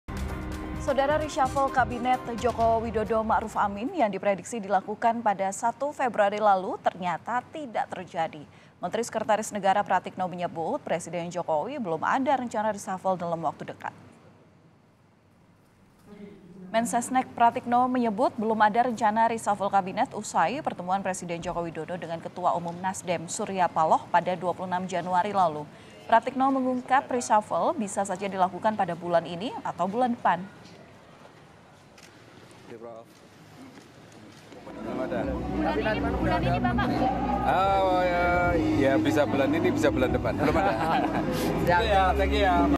Ditanya Soal Reshuffle Kabinet Jokowi, Ini Jawaban Mensesneg Pratikno!
JAKARTA, KOMPAS.TV - Reshuffle Kabinet Joko Widodo-Maruf Amin, yang diprediksi dilakukan 1 Februari lalu, ternyata tak terjadi.

Menteri Sekretaris Negara, Pratikno menyebut, Presiden Jokowi belum ada rencana reshuffle dalam waktu dekat.

Mensesneg, Pratikno menyebut, belum ada rencana reshuffle kabinet usai pertemuan Presiden Joko Widodo, dengan Ketua Umum Nasdem, Surya Paloh, 26 Januari 2023 lalu.

Pratikno mengungkapkan, reshuffle bisa saja dilakukan pada bulan ini, atau bulan depan.

Setelah sebeulumnya Jokowi memberi sinyal, meskipun secara umum kinerja kabinetnya baik, namun ada pos-pos kementerian yang butuh evaluasi.

Presiden pun tak menampik, selain kinerja, faktor politik tak bisa dilepaskan dari keputusan "reshuffle" yang diambil.